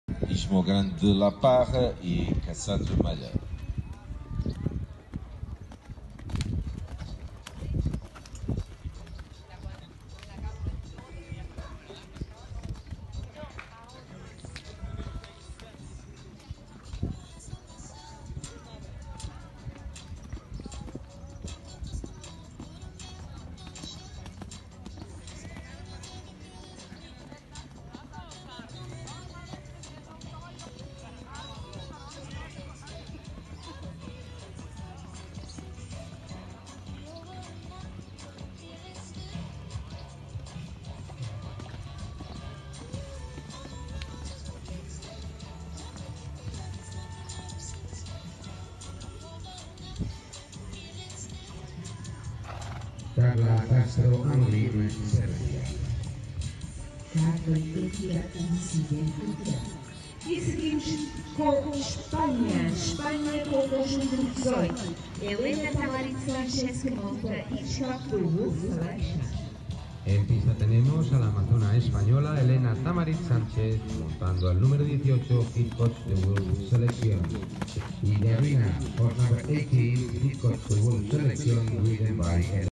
Esmugram de la Parra caballo en venta
NOMBRE: GAVIOTA DE LA PARRA
RAZA: Anglo-árabe
Padre: YARTAN DE LA PARRA
Madre: ZURITA DE LA PARRA
Abuelo Materno: CRIPTANO II (PSI)

Este potro es hijo de Yartan de la Parra y Zurita por Criptano XX. Es fruto del inbreeding 3×4 con la yegua PSI Criptana.

Por su montabilidad y buena cabeza, pensamos que va a ser un buen saltador para un joven amateur y/o un excelente caballo para el concurso completo, pues tiene un gran tranco de galope.

Actualmente está realizando pruebas de 1,10m y CCE, nivel 0*.